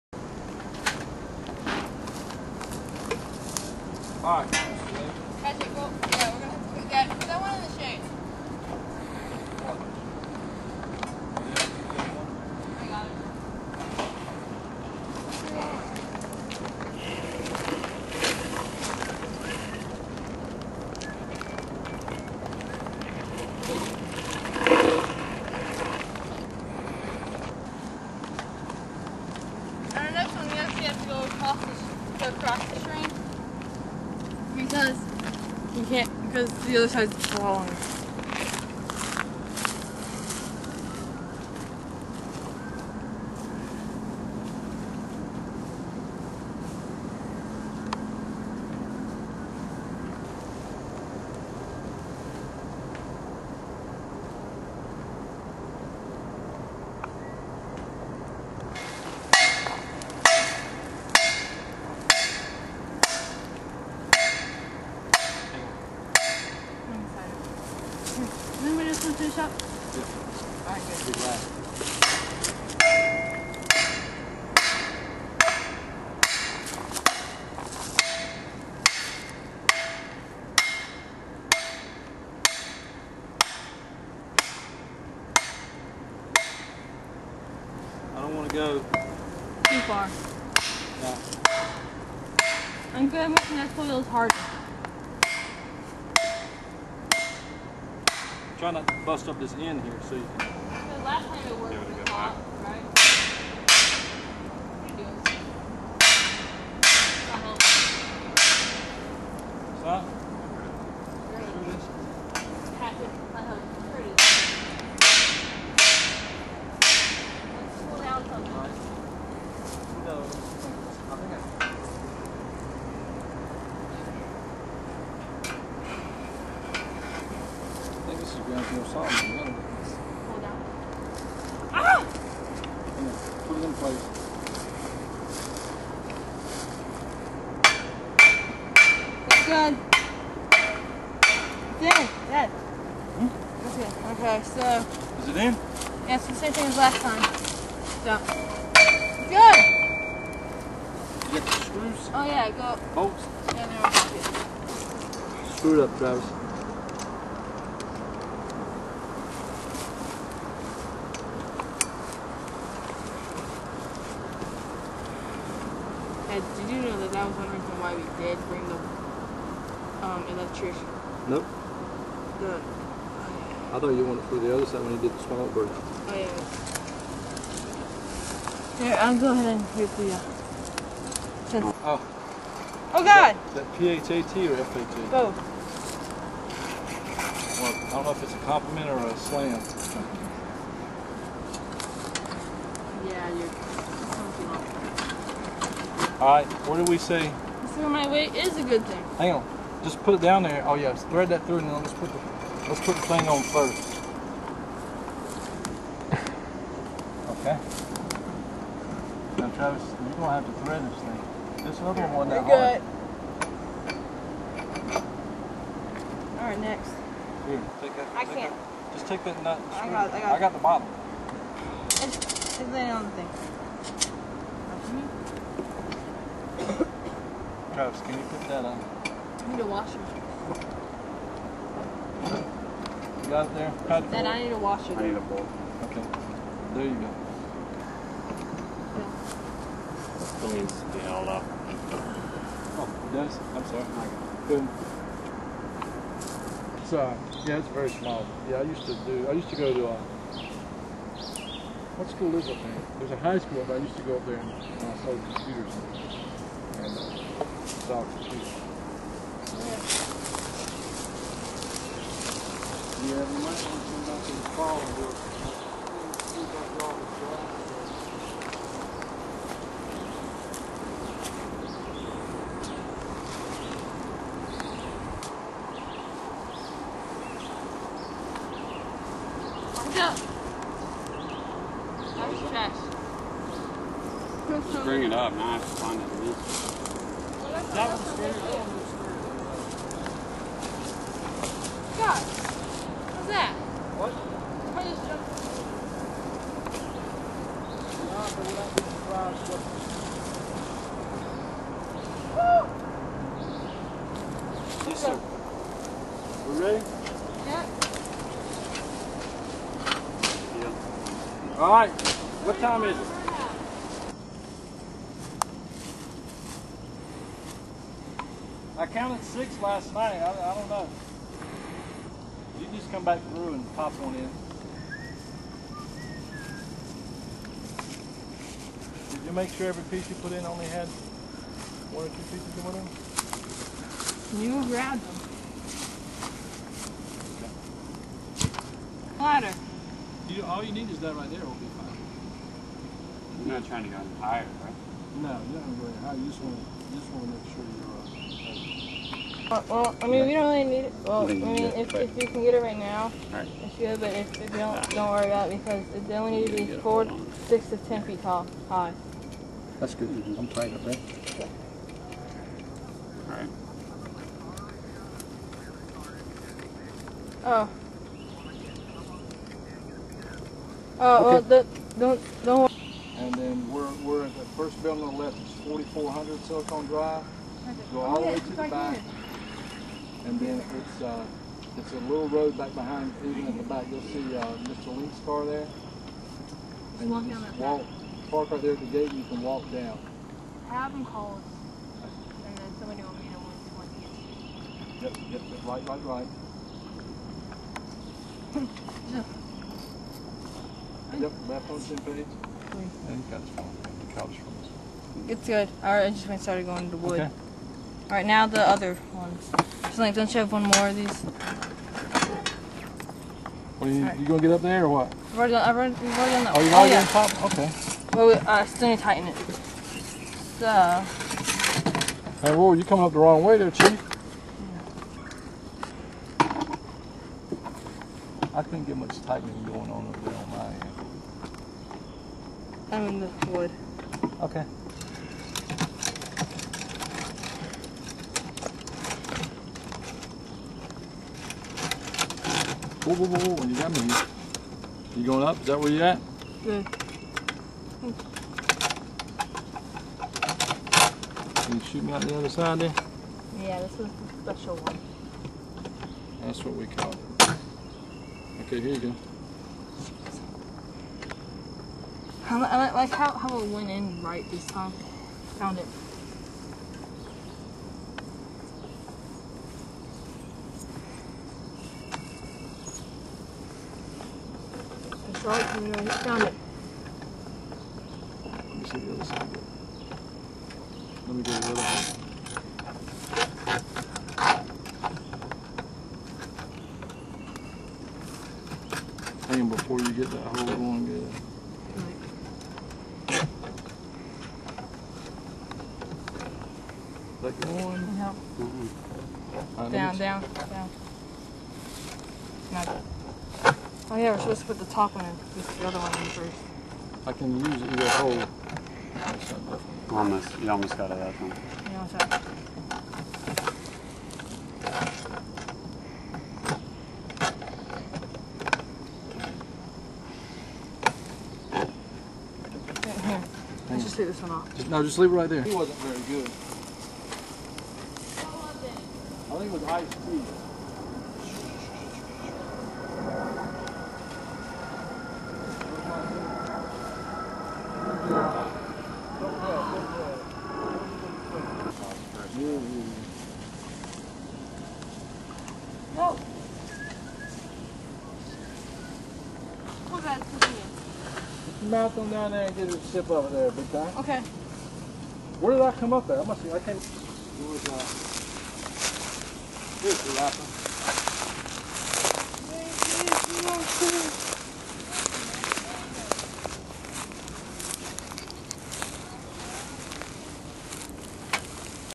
Travis Johnston Eagle Project 2009/05/30 Part 2
Part 2 of 3 installation of the bird houses in the RTP wilderness